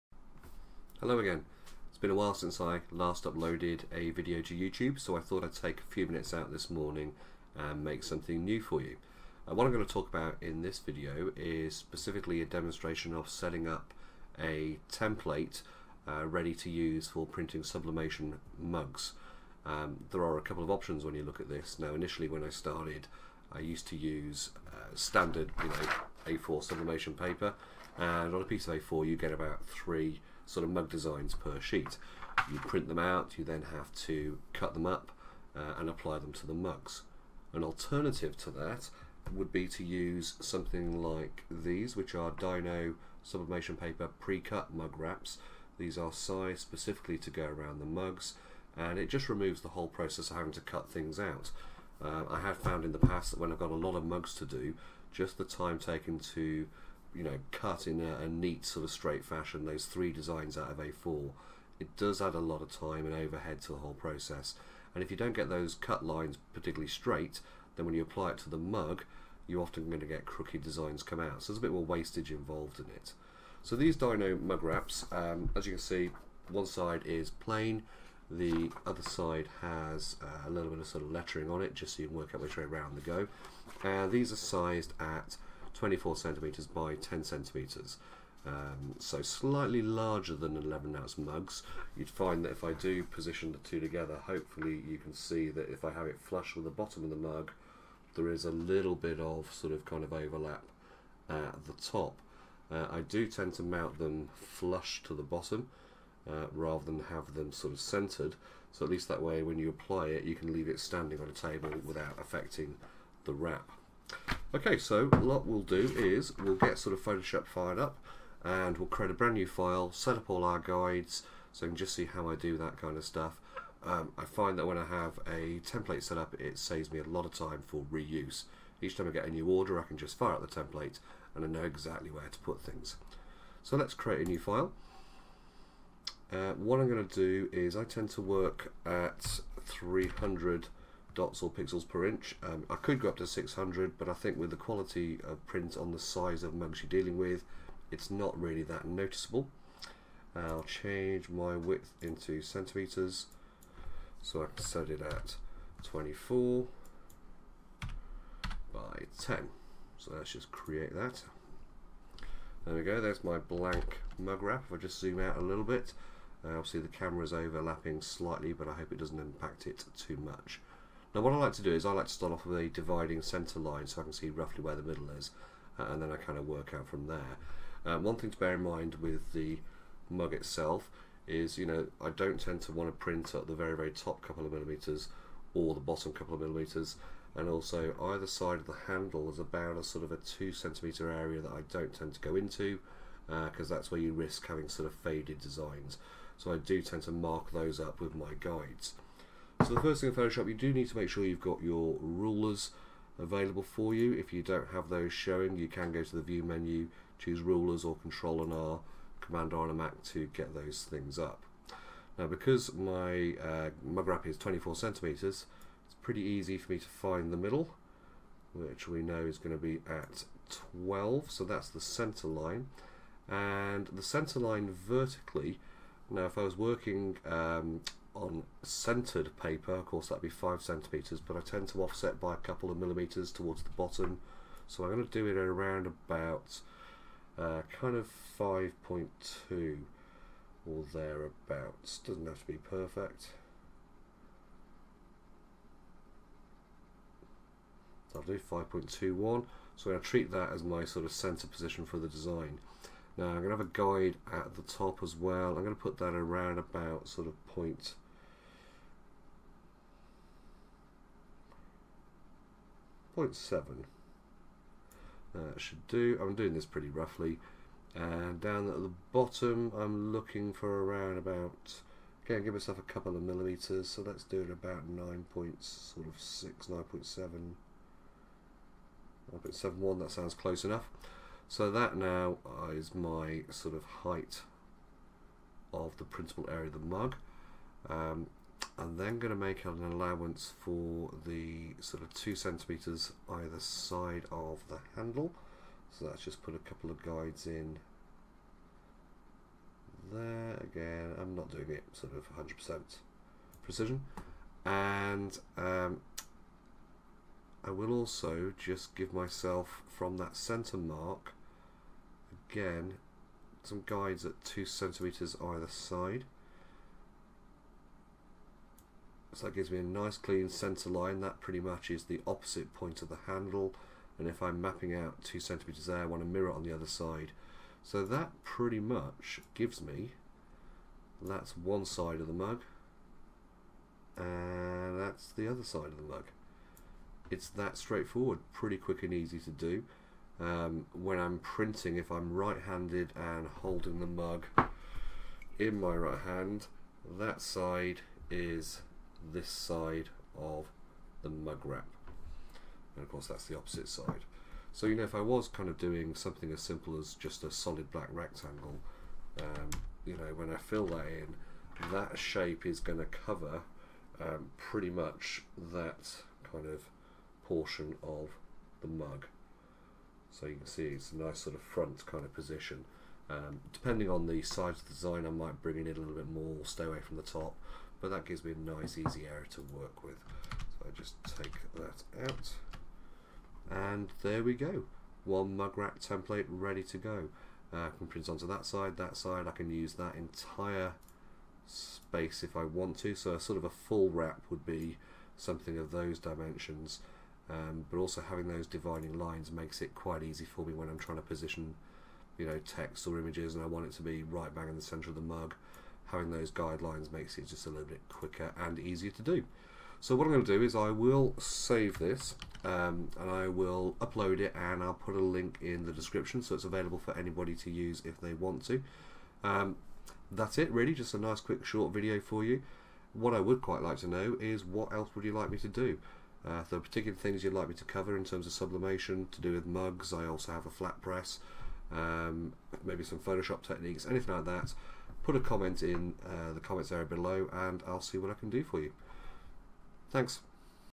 Dino pre-cut sublimation mugwraps - making a Photoshop template
In this short video, I run through how I make up a template in Photoshop for use with Dino Sublimation Mugwraps (pre-cut).  The wraps are sized at 240mm x 100mm.

You can download a copy of the PSD template file